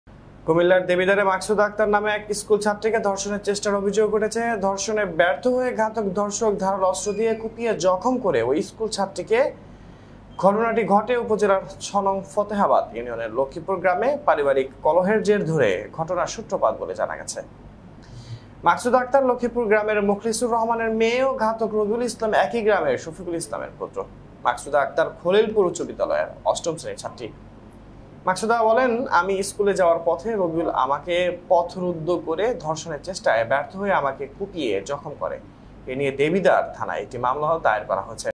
দেবিদ্বারে স্কুল ছাত্রীকে ধর্ষণের চেষ্টা || 71bangla Tv
কুমিল্লার দেবিদ্বারে মাকসুদা আক্তার নামে এক স্কুল ছাত্রীকে ধর্ষণের চেষ্টার অভিযোগ উঠেছে, ধর্ষণে ব্যর্থ হয়ে ঘাতক ধর্ষক ধারালো অস্ত্র দিয়ে কুপিয়ে জখম করে ওই স্কুল ছাত্রীকে, ঘটনাটি ঘটে উপজেলার ৬নং ফতেহাবাদ ইউনিয়নের লক্ষীপুর গ্রামে, পারিবারিক কলহের জের ধরে ঘটনার সূত্রপাত বলে জানা গেছে, মাকসুদা আক্তার লক্ষীপুর গ্রামের মোকলেছুর রহমানের মেয়ে ও ঘাতক রবিউল ইসলাম একই গ্রামের শফিকুল ইসলামের পুত্র। মাকসুদা আক্তার খলিলপুর উচ্চ বিদ্যালয়ের ৮ম